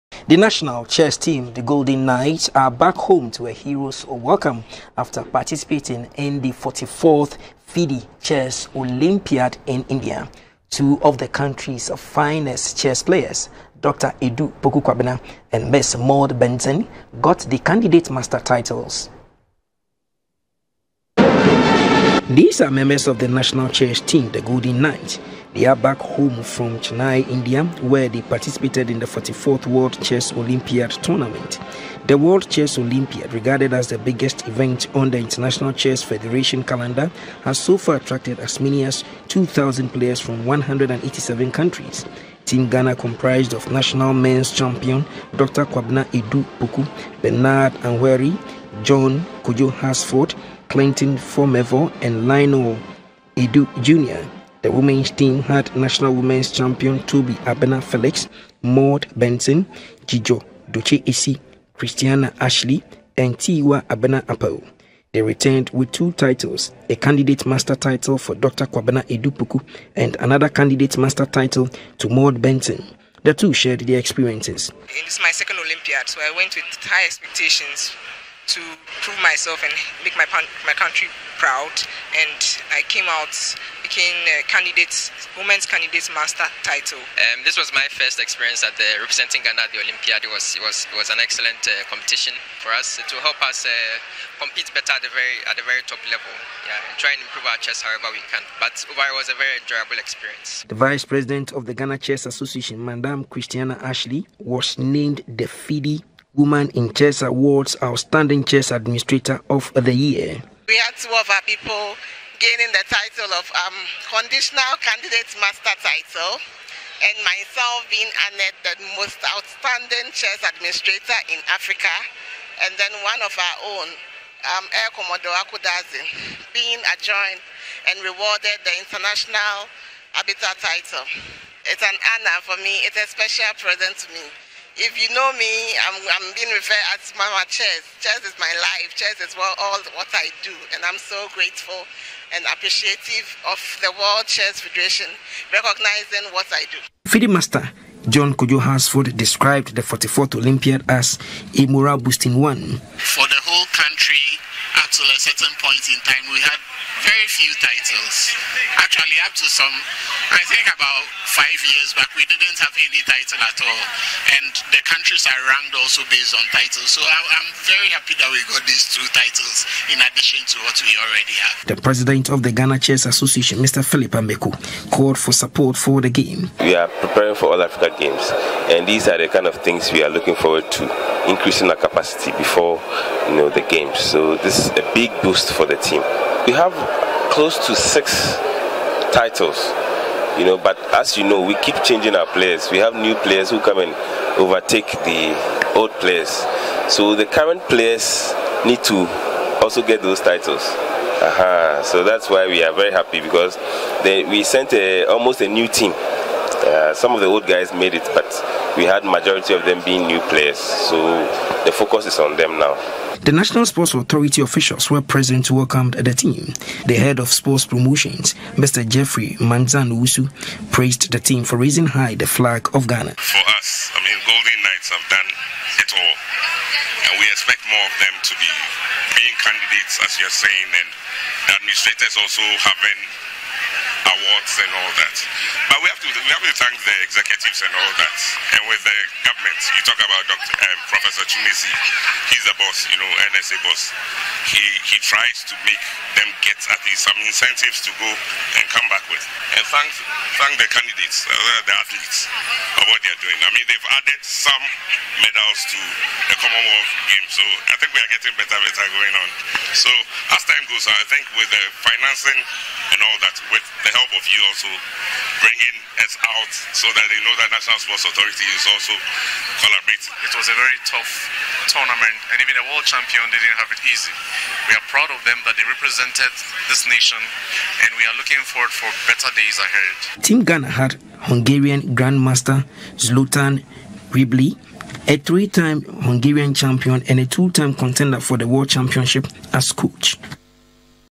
GHANA'S CHESS TEAM RETURNS FROM 44TH OLYMPIAD
The national Chess team, the Golden Knights, are back home to a hero’s welcome after participating in the 44th FIDE Chess Olympiad in India. Two of the country’s finest chess players, Dr Adu Poku Kwabena and Miss Maud Benson got the Candidate Master titles.